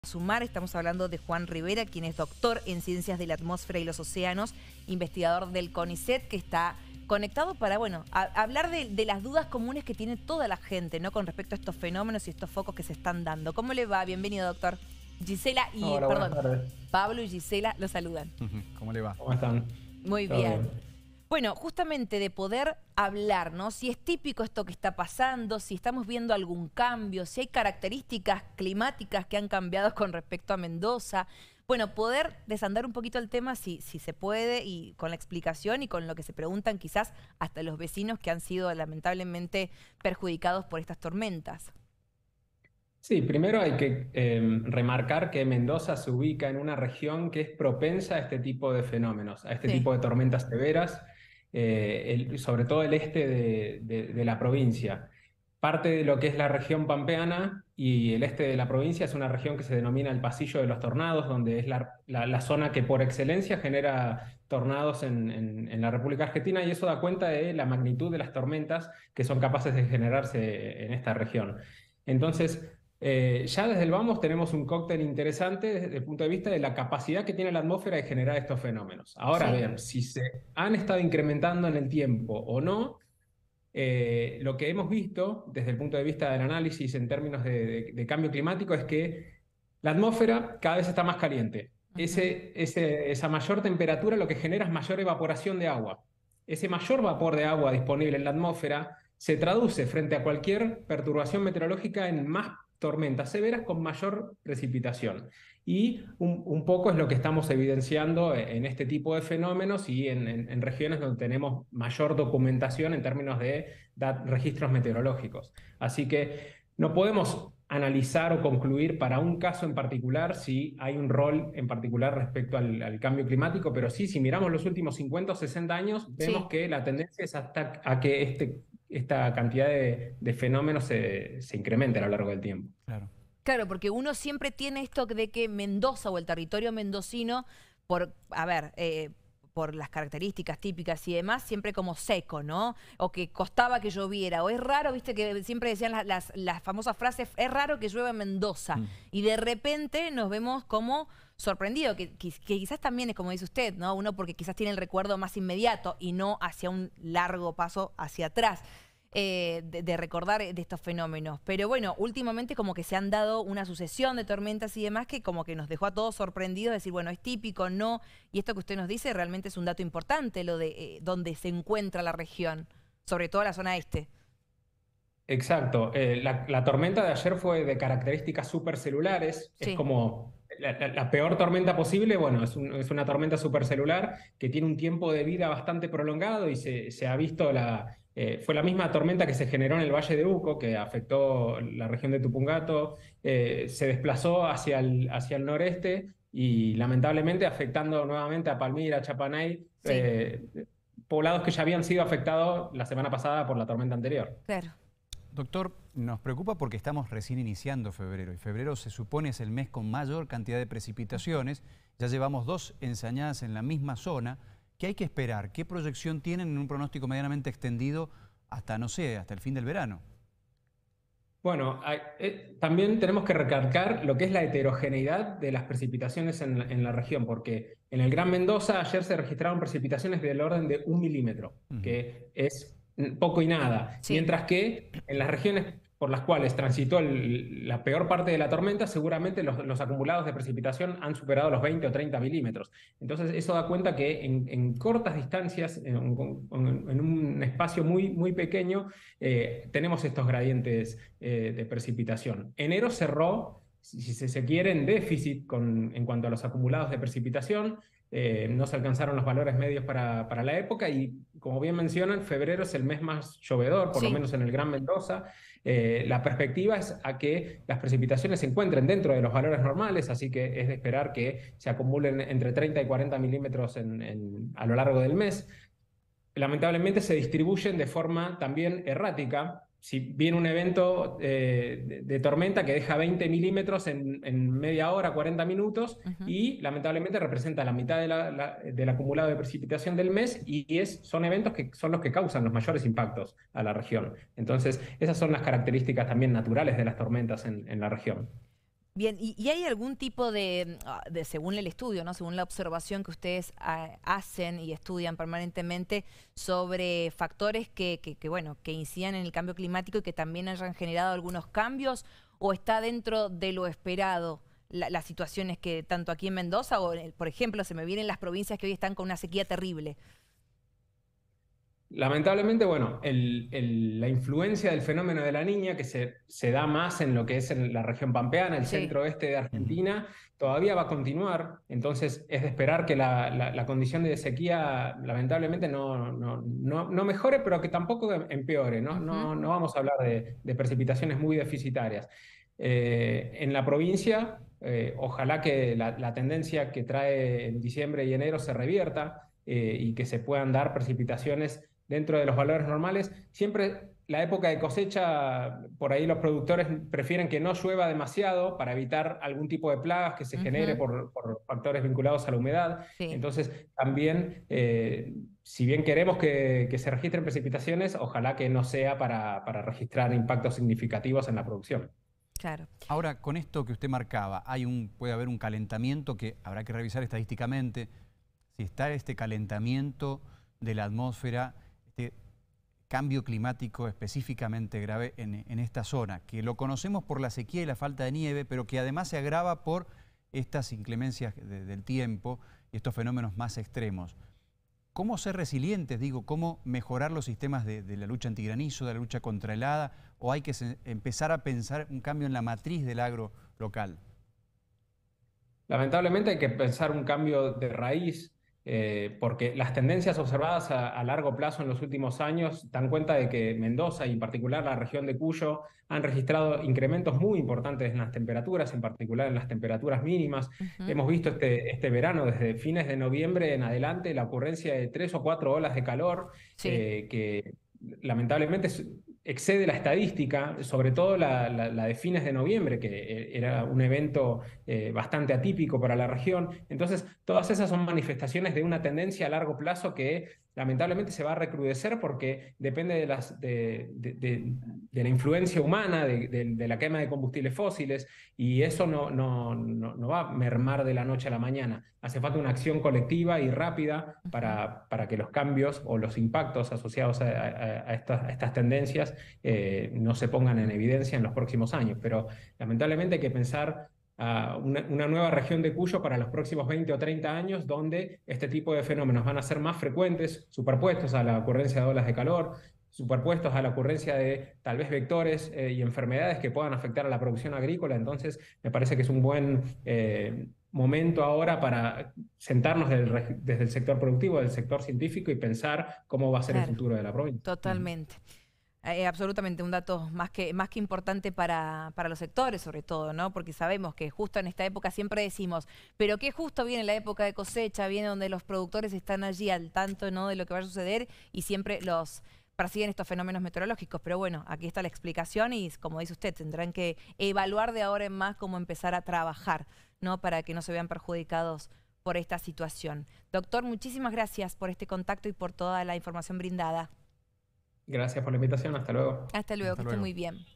PREOCUPA LA CANTIDAD DE FOCOS TORMENTOSOS
Dialogamos con Juan Rivera, doctor en Ciencias de la Atmósfera y los Océanos Instituto Argentino de Nivología, Glaciología y Ciencias Ambientales del CONICET.